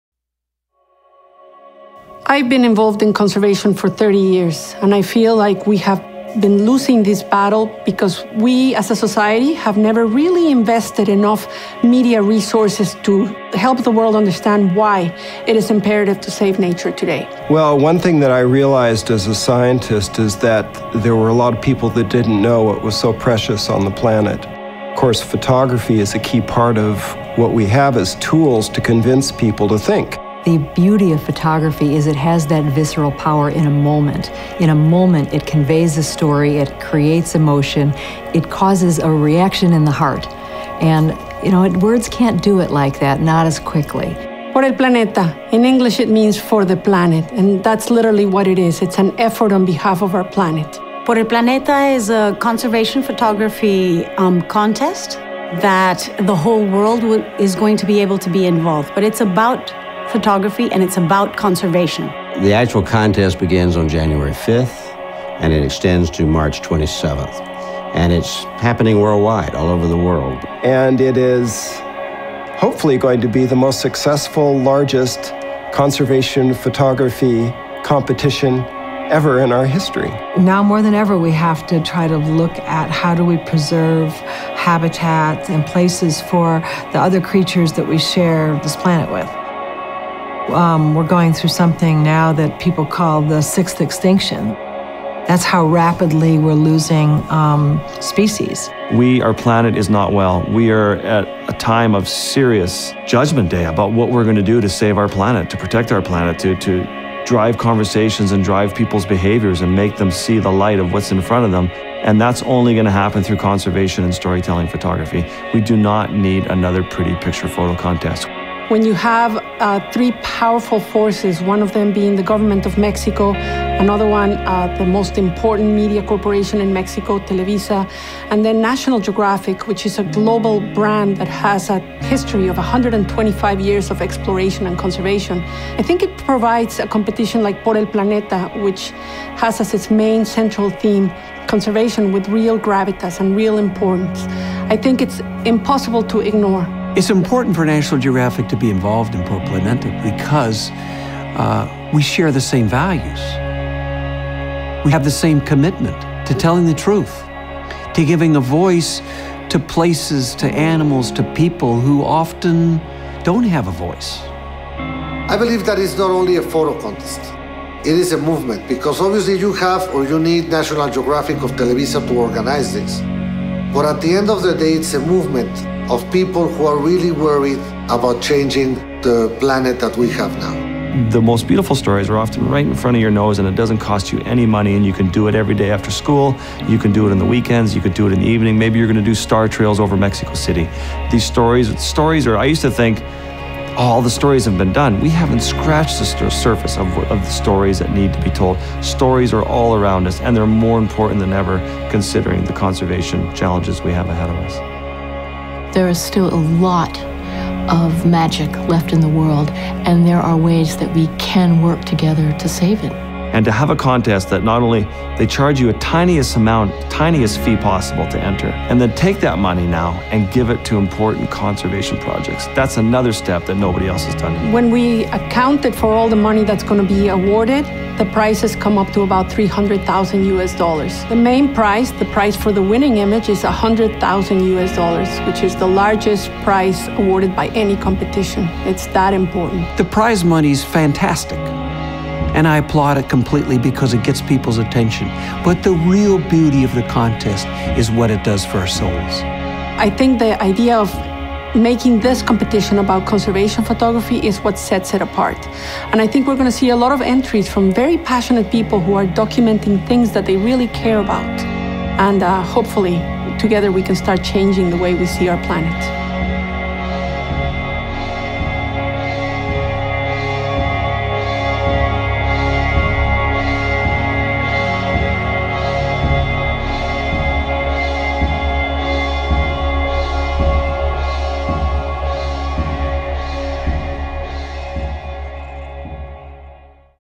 Por El Planeta - Entrevistas (Español)
La fotografía es una de las herramientas más eficaces que podemos utilizar para convencer a la sociedad de que es el momento idóneo para comenzar a actuar por nuestro planeta.  Hoy, fotógrafos de todo el mundo,@Televisa.com, @National Geographic, @Sectur y @ConacultaDGP se unen en con el fin de transformar nuestro planeta en: Por El Planeta, Wildlife, Nature & Conservation Photo Competition, el concurso internacional de fotografía de conservación más importante del 2015.
Sube tus imágenes en porelplanetaphoto.com y sé parte de esta comunidad en pro de la conservación.